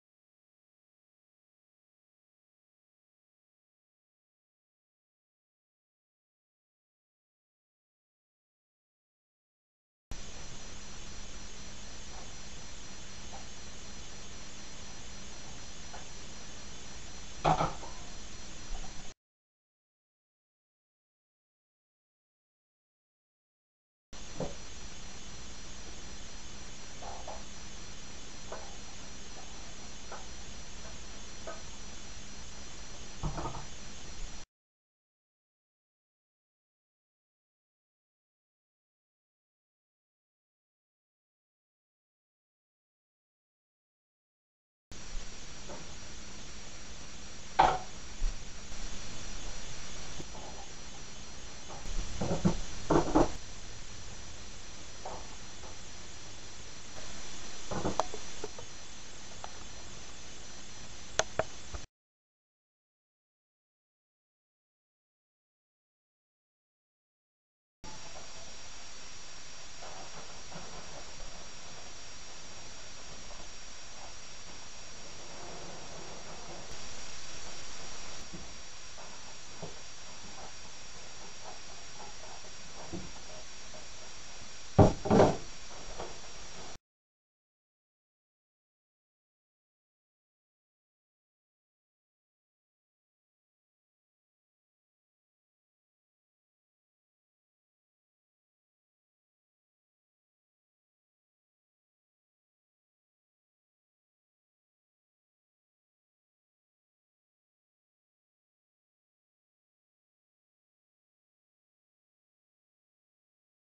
The Pourable Colander silent Movie.wmv
New colander designed specifically for pouring fruits, vegetables and pasta. This new colander features two large handles for control, tapered pour spout to reduce spilling, multiple long slots for faster draining and wide base for stability. The Pourable Colander is a kitchen must have utensil.